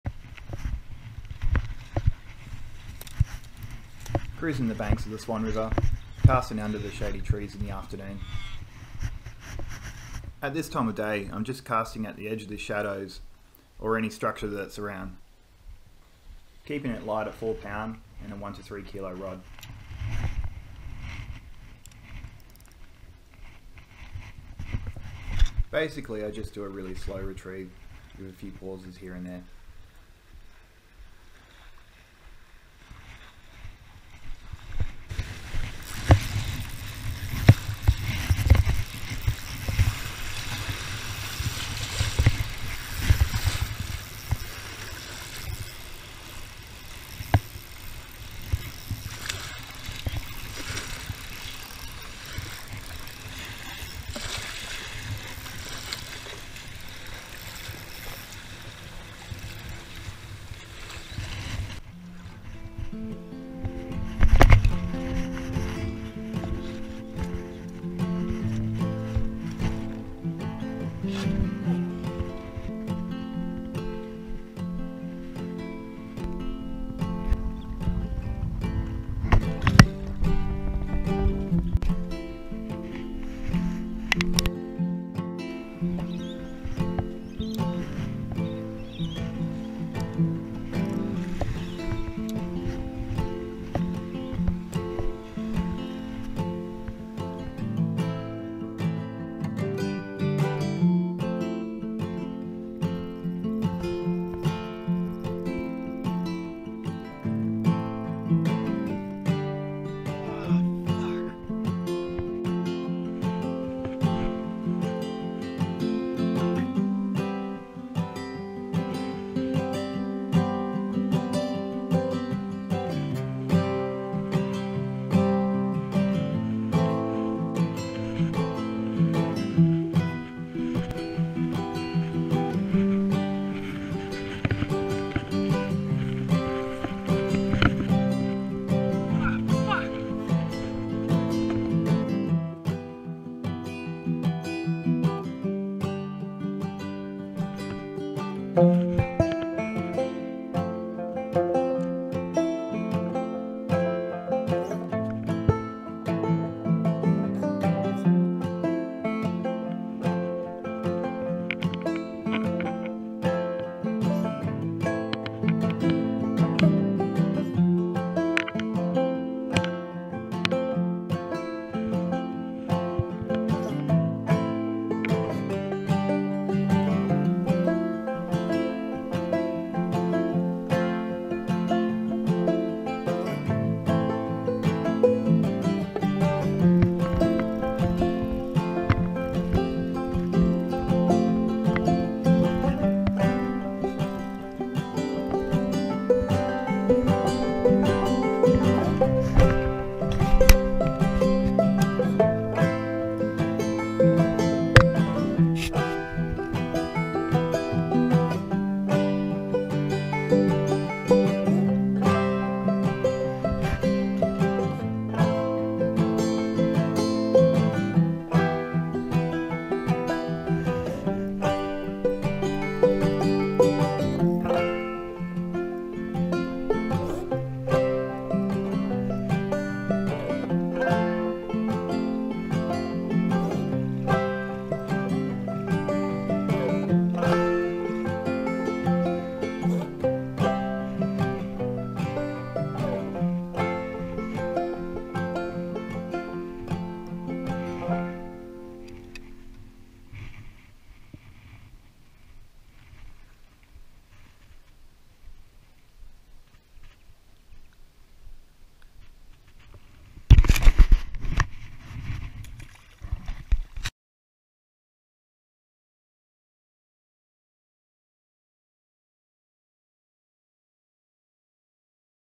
Fishing the Shade Line for Black Bream - Swan River WA
Fishing the shade and structure land based for bream. Walking along the banks and rock walls flicking soft plastics on the shade line with a slow retrieve. Got a nice donkey bream and a bunch of smaller numbers. Also got my lure chomped of by a decent Tailor for river standards.